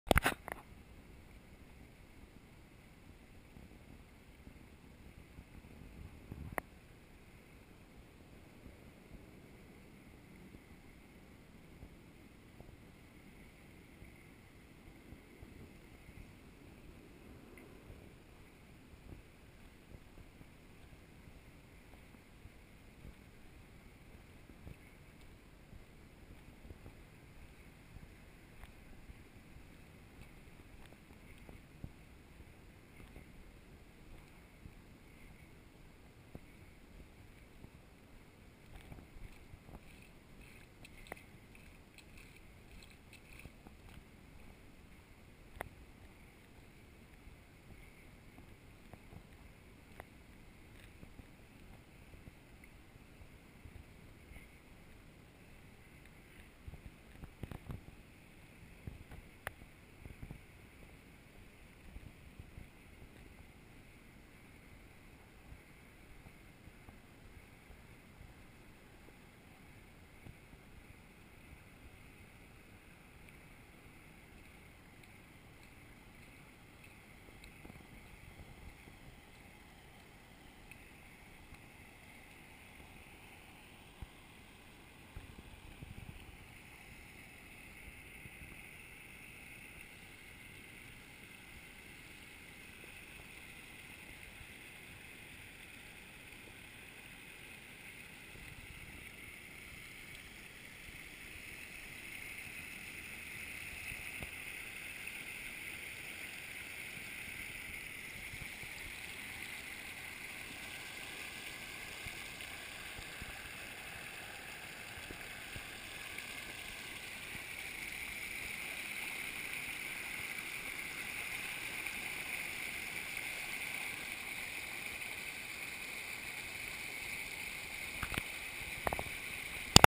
Campbell Creek Flooding In Anchorage Alaska
Flooding along the Campbell Creek Trail just north of Diamond Boulevard in Anchorage Alaska.  Usually the creek is low enough to walk across.